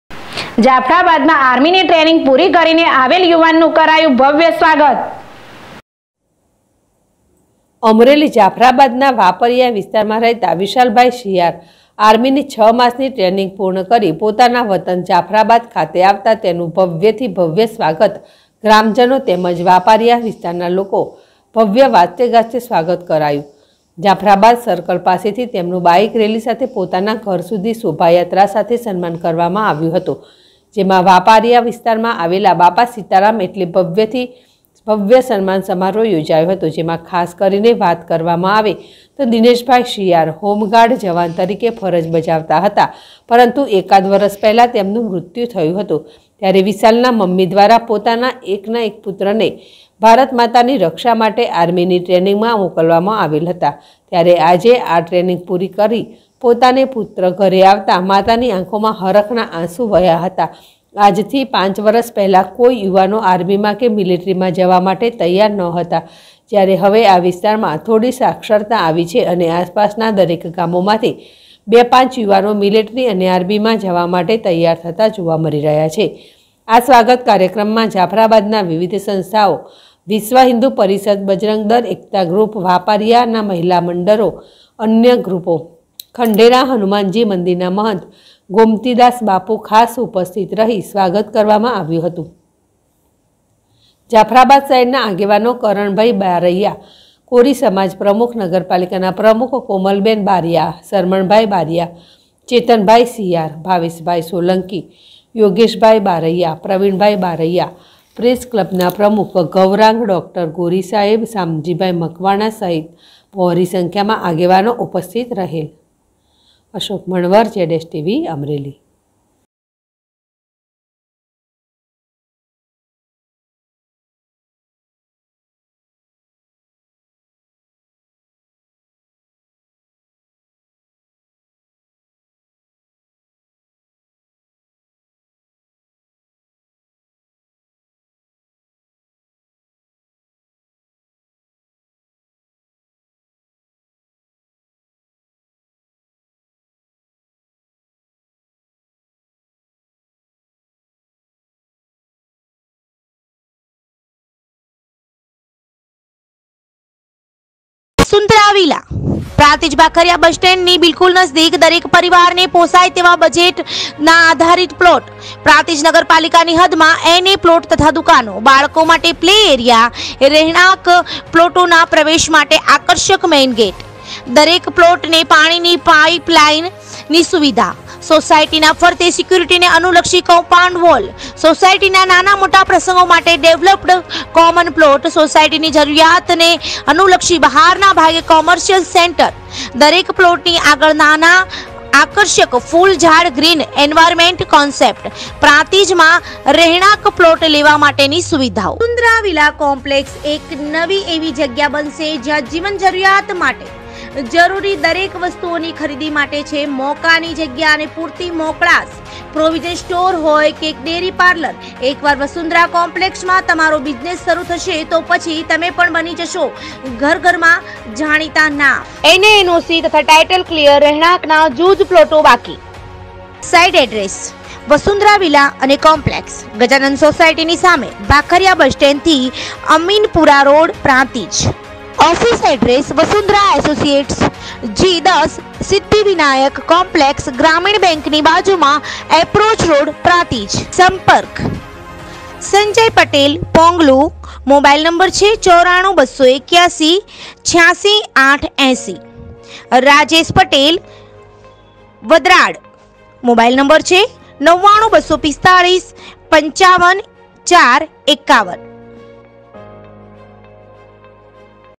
જાફરાબાદમાં આર્મીની ટ્રેનિંગ પૂરી કરીને આવેલ યુવાનનું કરાયું ભવ્ય સ્વાગત કરાયું...
જાફરાબાદમાં આર્મીની ટ્રેનિંગ પૂરી કરીને આવેલ યુવાનનું કરાયું ભવ્ય સ્વાગત કરાયું.//ZSTVNEWS//